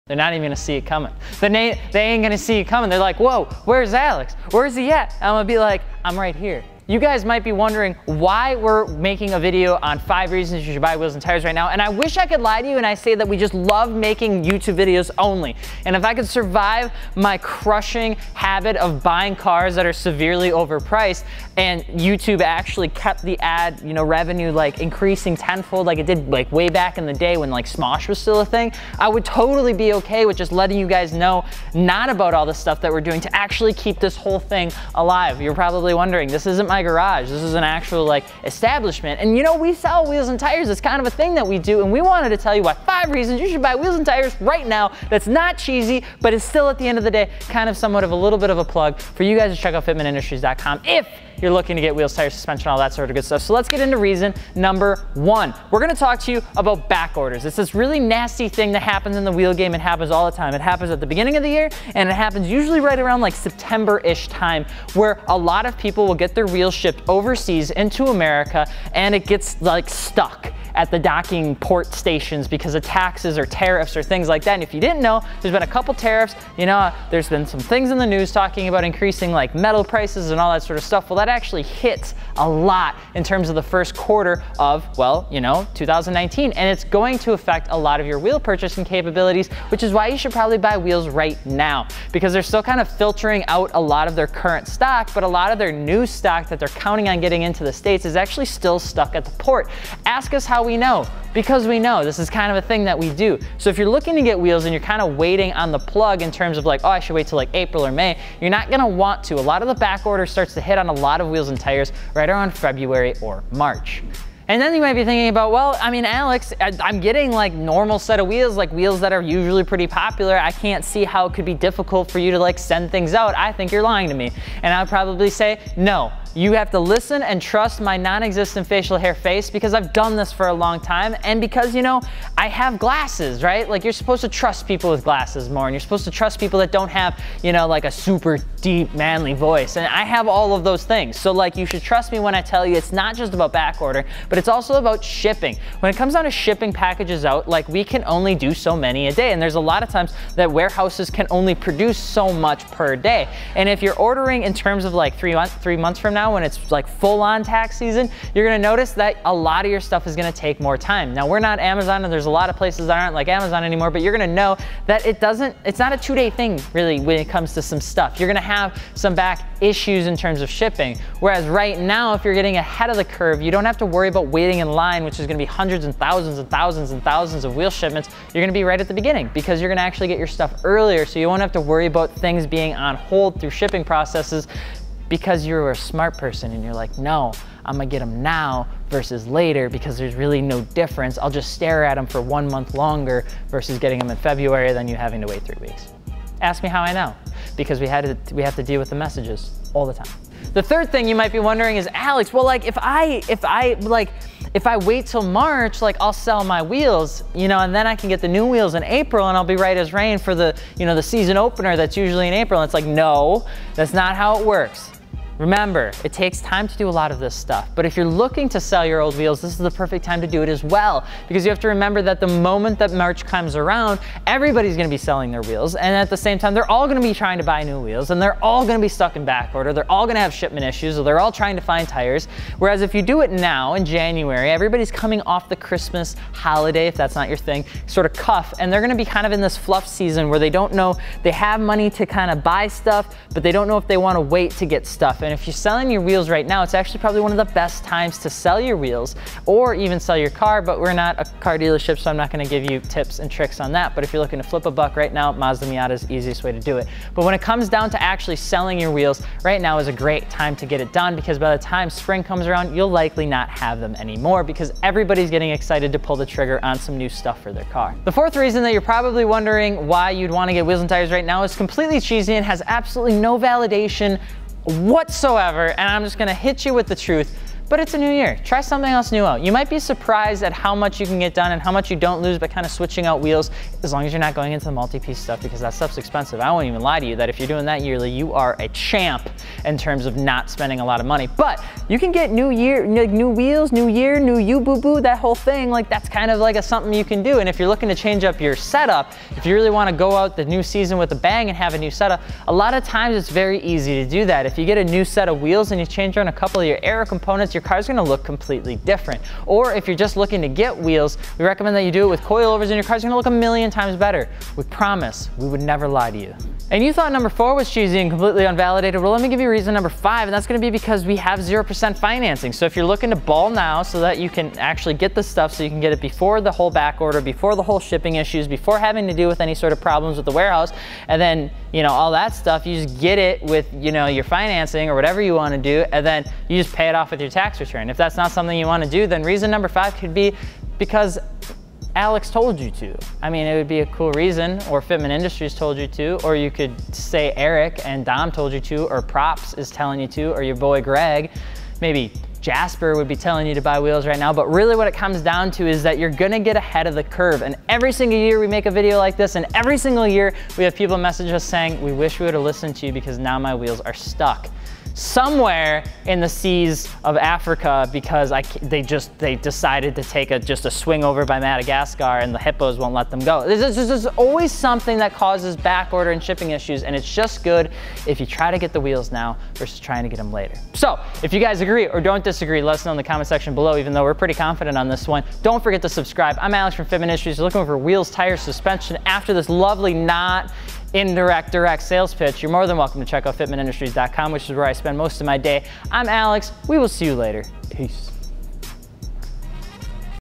Why You Should Buy Car Parts Right Now!
Have you had your eyes on new wheels or the latest FI apparel? Well today is your lucky day, because we give you 5 reason why you should buy wheels and tires from Fitment Industries right now!  No pressure though...well maybe a little.

We’re offering as low as 0% financing for the whole month of January!